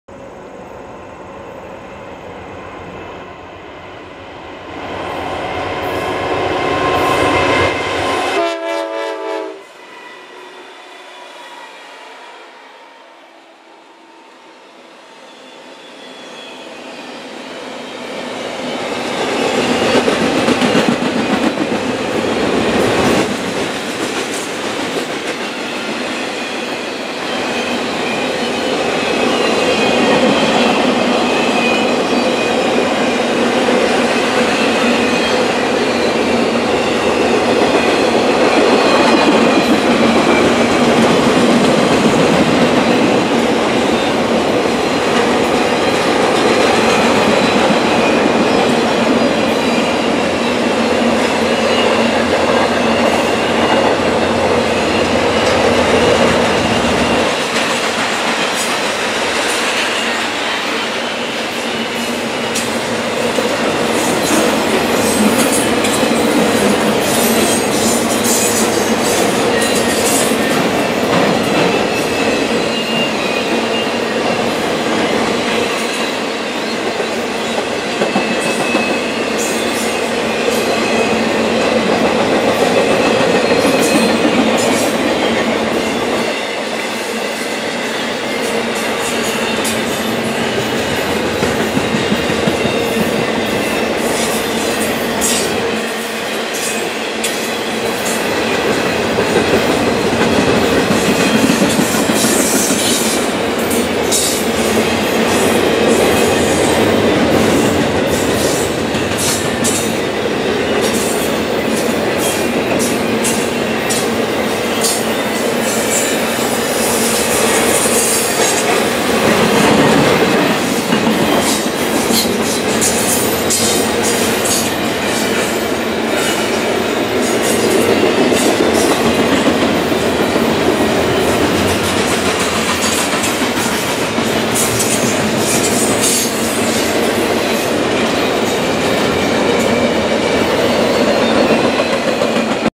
NS 211
NS 211 heading towards Chambersburg on the NS Lurgan Branch.